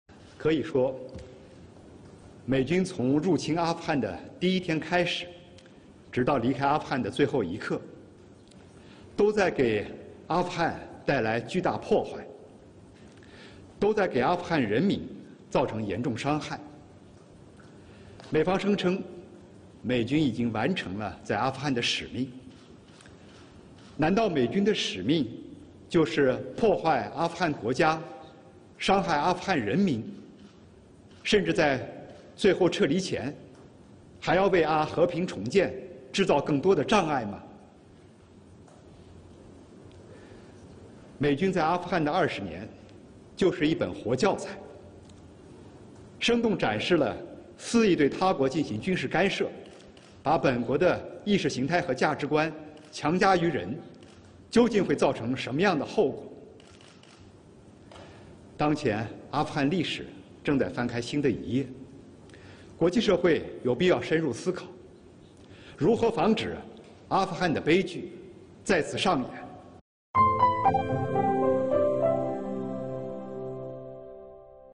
汪文斌：美军在阿富汗的20年就是一本军事干涉他国的“活教材”
中国外交部发言人汪文斌9月8日在例行记者会上指出，美军在阿富汗的20年就是一本军事干涉他国的“活教材”。

有记者提问，有报道称，美军完成从阿富汗撤离前，故意毁坏了喀布尔机场雷达等设施。美国中央司令部司令麦肯齐承认，美军离开喀布尔机场之前破坏了机场内的数10架飞机，据了解这其中包括阿富汗的民航飞机，请问中方对此有何评论？

汪文斌表示，“可以说美军从入侵阿富汗的第一天开始，直到离开阿富汗的最后一刻，都在给阿富汗带来巨大破坏，都在给阿富汗人民造成严重伤害。”

汪文斌指出，美方声称“美军已经完成了在阿富汗的使命”，难道美军的使命就是破坏阿富汗国家，伤害阿富汗人民，甚至在最后撤离前，还要为阿和平重建制造更多的障碍？美军在阿富汗的20年，就是一本活教材，生动展示了肆意对他国进行军事干涉，把本国的意识形态和价值观强加于人，究竟会造成什么样的后果。

汪文斌说，当前阿富汗历史正在翻开新的一页，国际社会有必要深入思考如何防止阿富汗的悲剧再次上演。